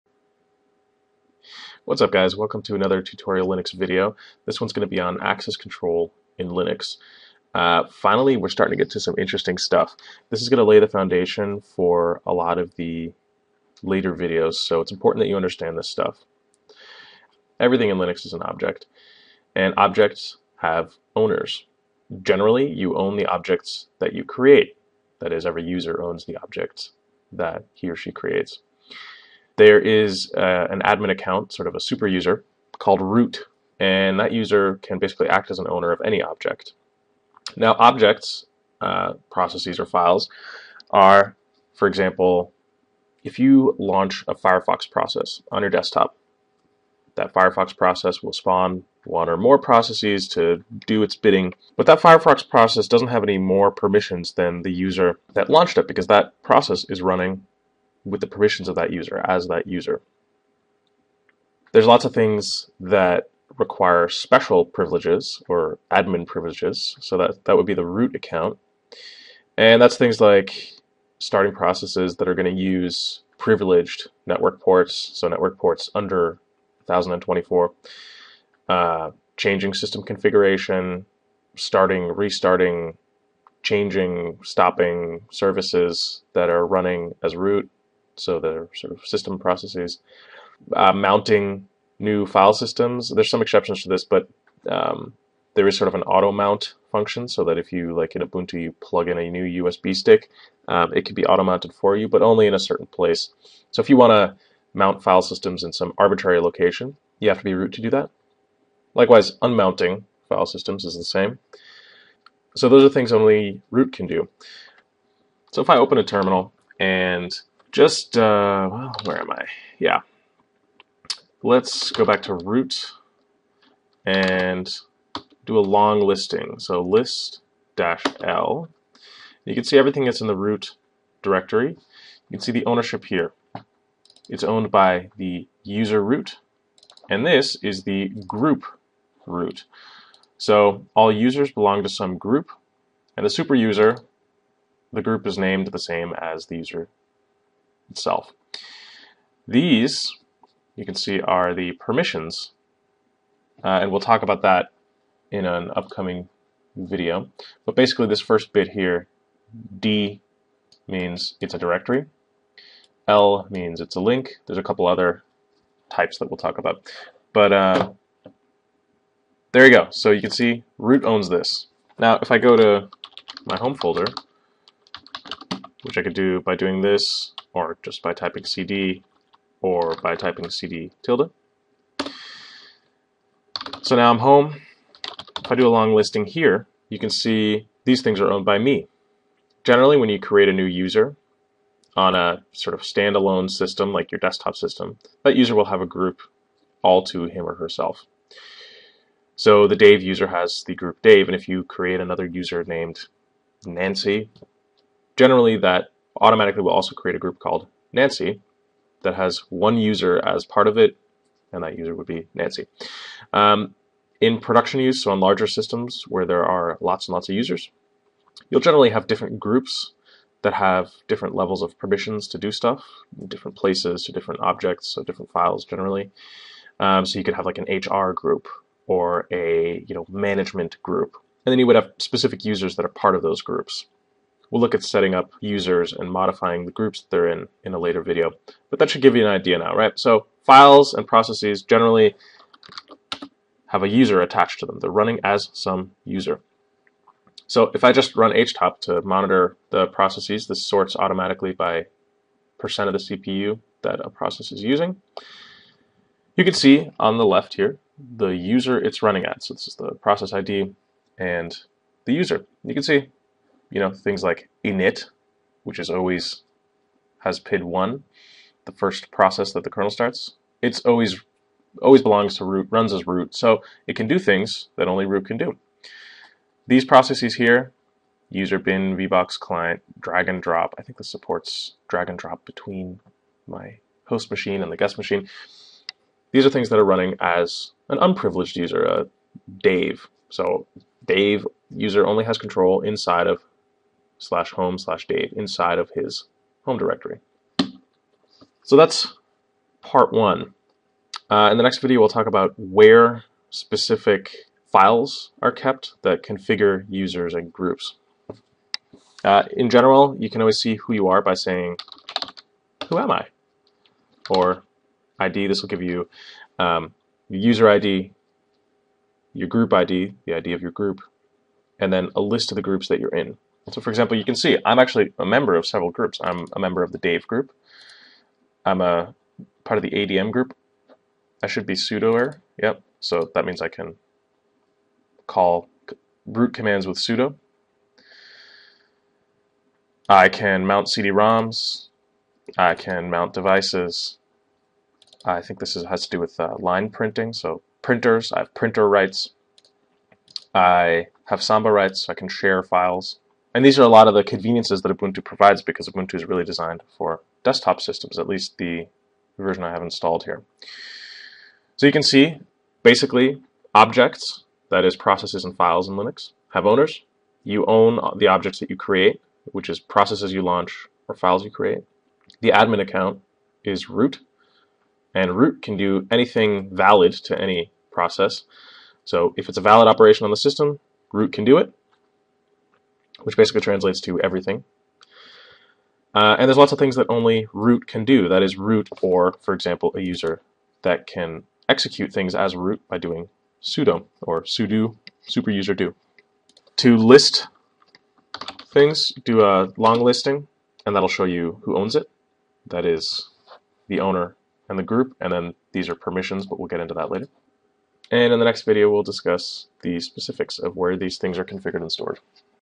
Basic Linux Access Control
How do Linux permissions work for processes? Which user owns a Linux process? How do Linux users get permission to access files and directories? How does access control work in Linux? In this video you'll get a quick intro on how Linux handles access control. We look at objects that users own (processes and files) and discuss some high-level concepts.

Useful commands:
# id
# whoami